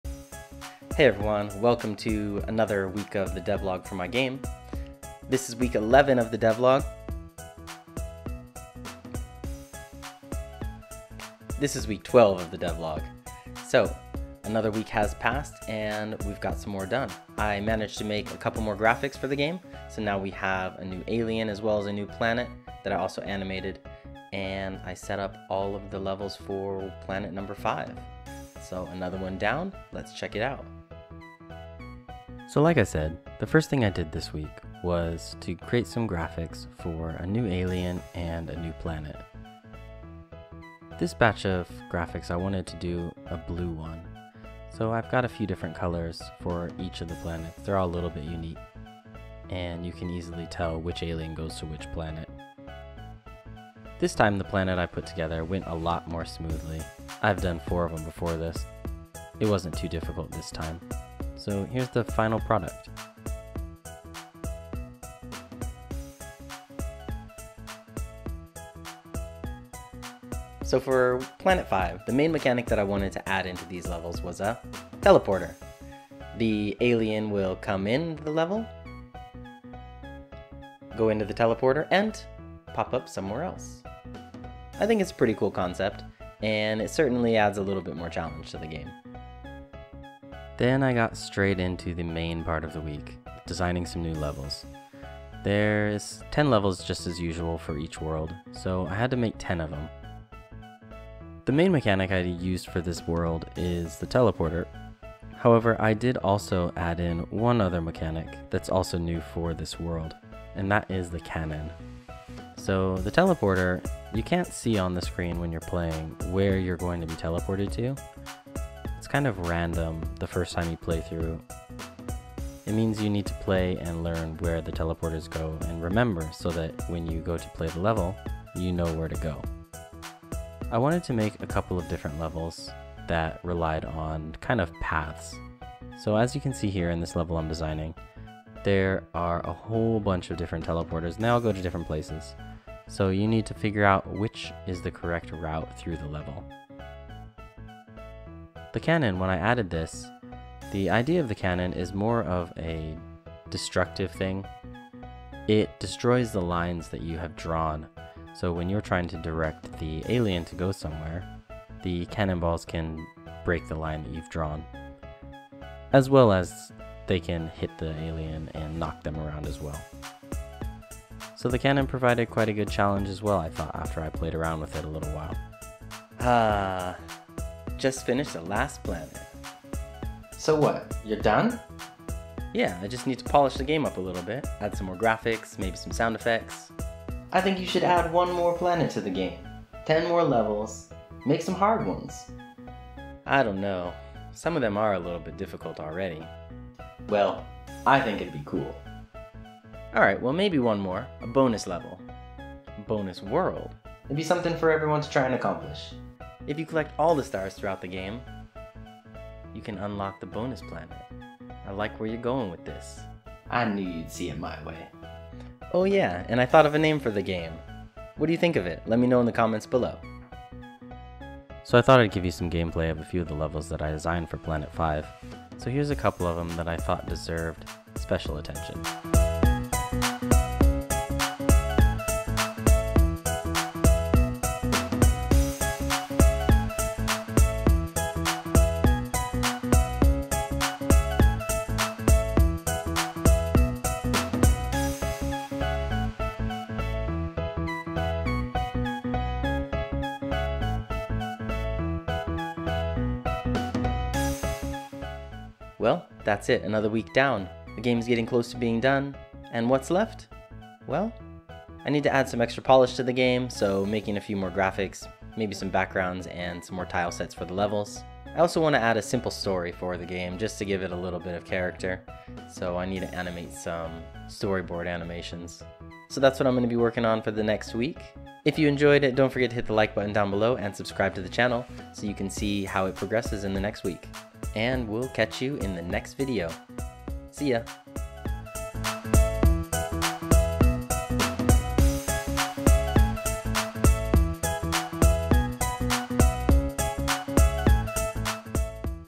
Dev Log - Final Planet - Episode 12
Creating levels for the final planet of the game. This planet uses the teleport and canons for obstacles. Not sure if it's the hardest of worlds but it is fun to play.

Let me know what you liked about the video or what you would like to see in the comments!